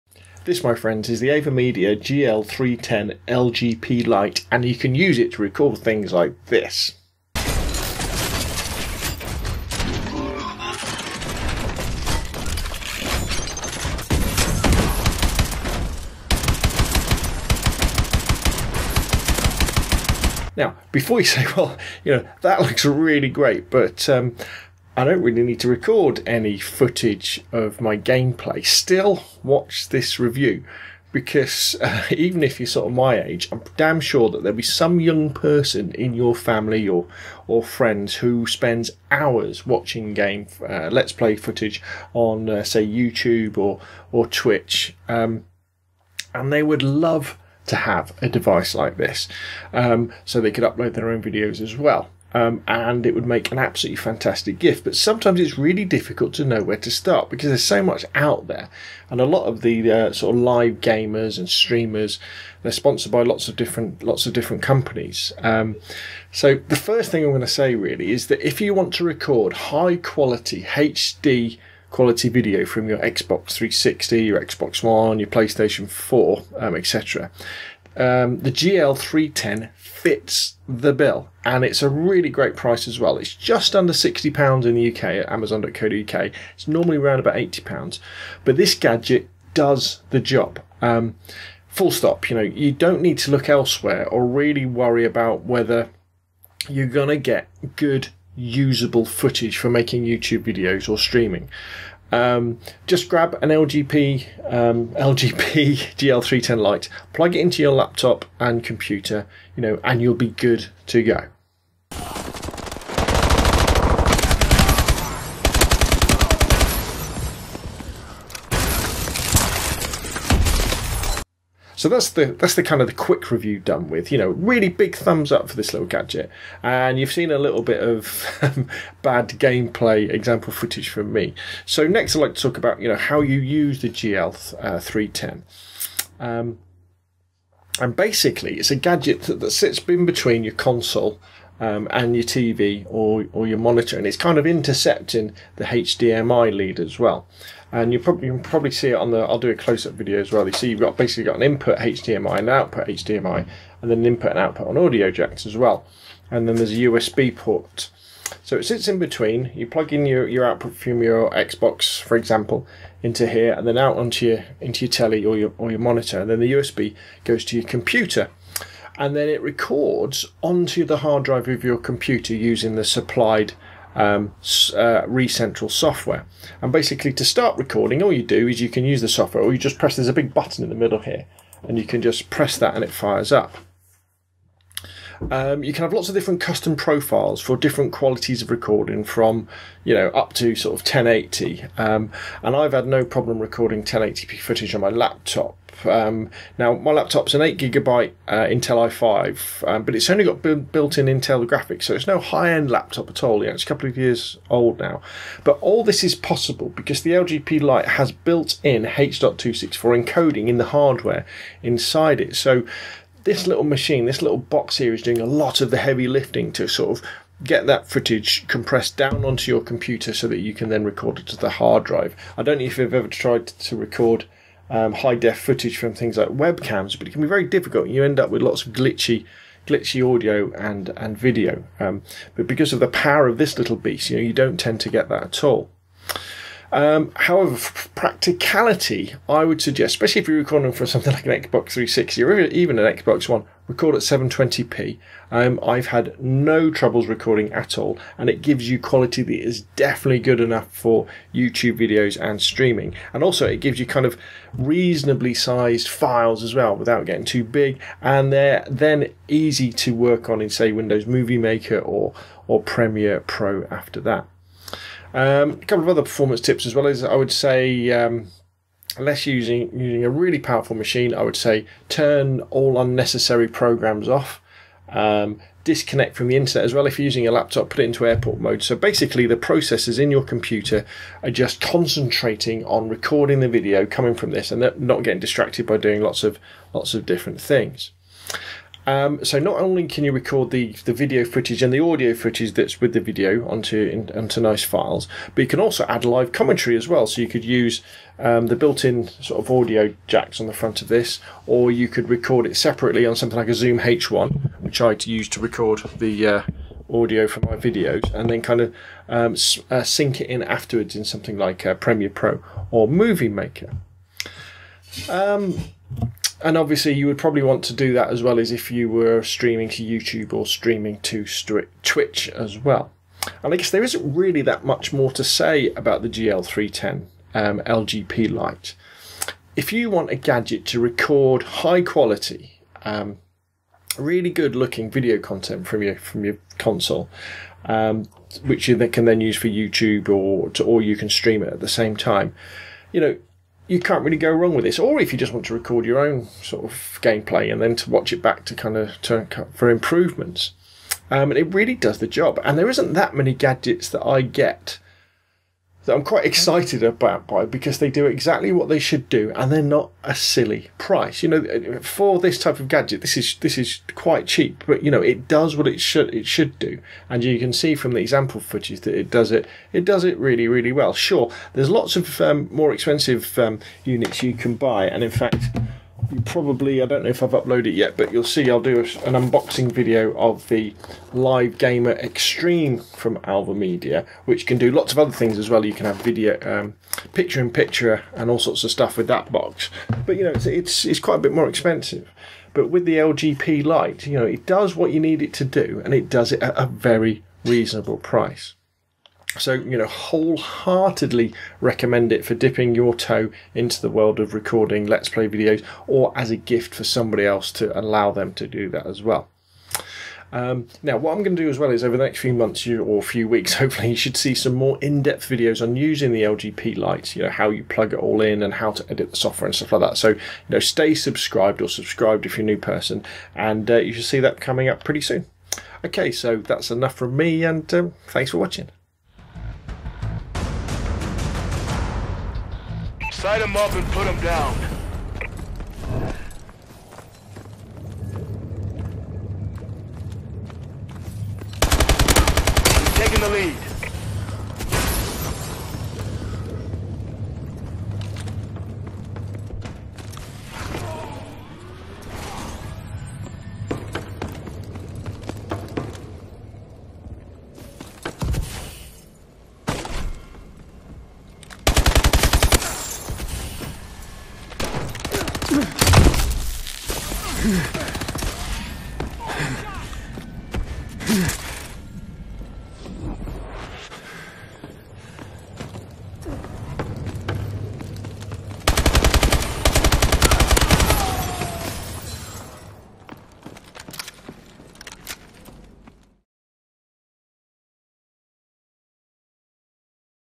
Avermedia LGP Lite GL310 Review HDMI Capture Box For Xbox 360 One & PS4
A fantastic little device at a great price that'll allow you to capture or stream HD footage from your game console. The Avermedia LGP Lite (GL310) is well within the budget of any gamer who wants to start uploading to youtube, and it's a great gift too!
Thanks, Rob.